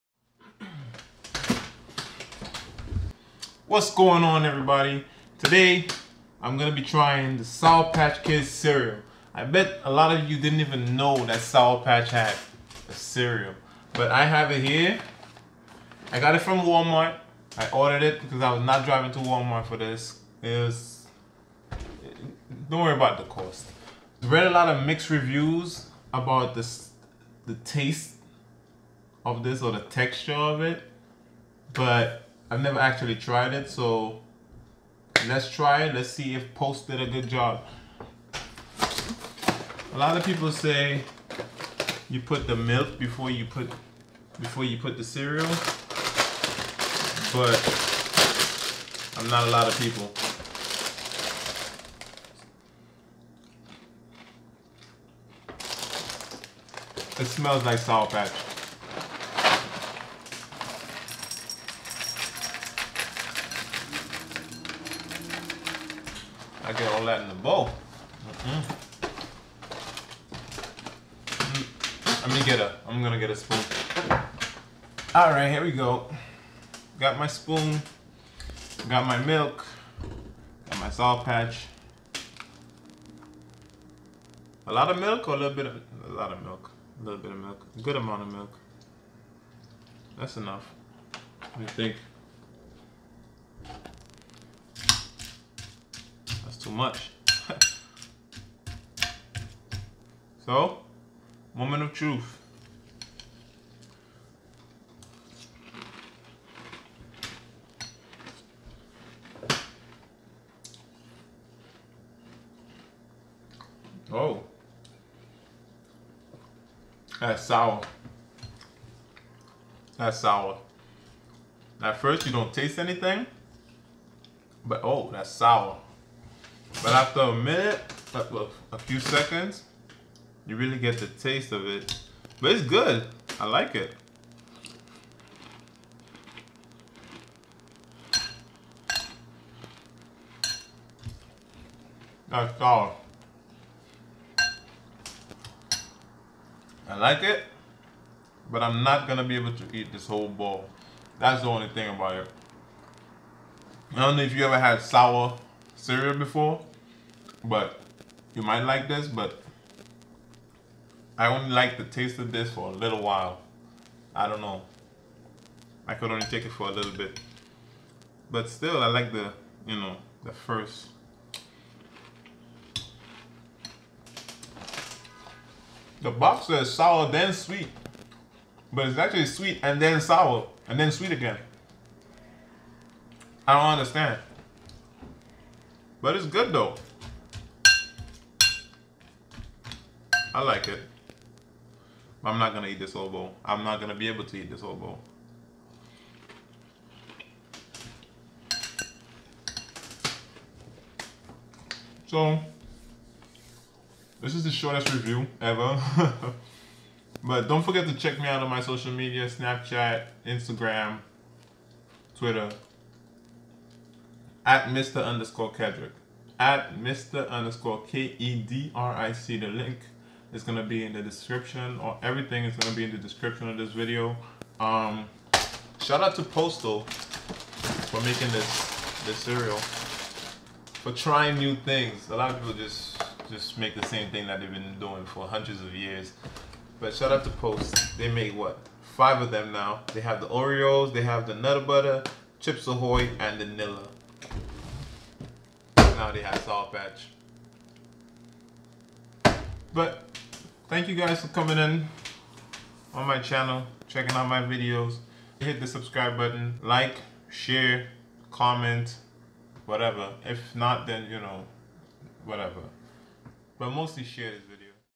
sour patch kids cereal review!
Today I got a box of cereal from Walmart.. yes it's the store patch kids cereal. Was not expecting much from it but check out the video for my reaction!